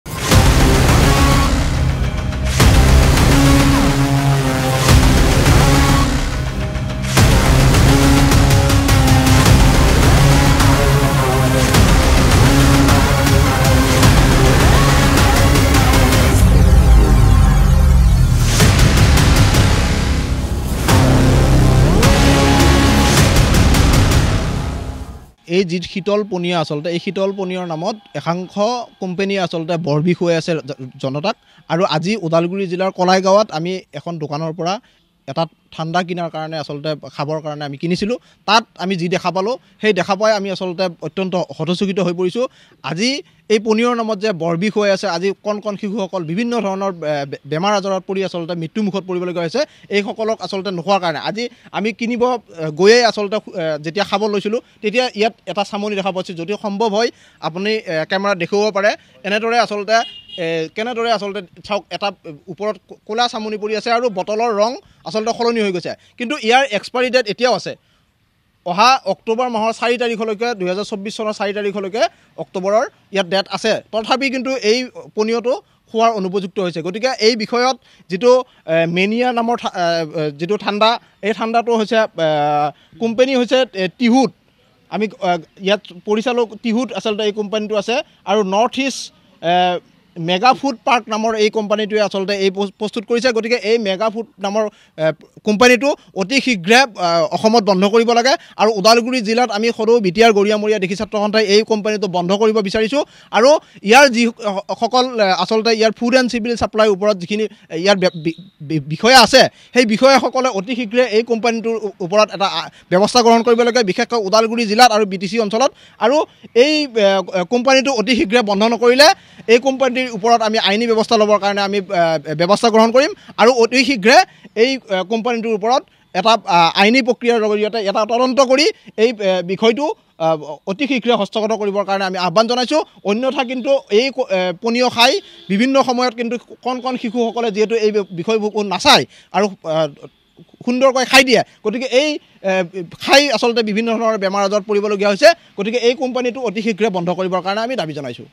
Udalguri News | সাৱধান। শীতল পানীয় 'মেনীয়া' বটলৰ ভিতৰত ওলাল ভয়াবহ কʼলা পদাৰ্থ। N18V
Udalguri News | সাৱধান। শীতল পানীয় 'মেনীয়া' বটলৰ ভিতৰত ওলাল ভয়াবহ কʼলা পদাৰ্থ। ঠাণ্ডা পানীয়ৰ নামত বৰবিষ খুৱাইছে এচাম পানীয় উৎপাদনকাৰী কোম্পানীয়ে। N18V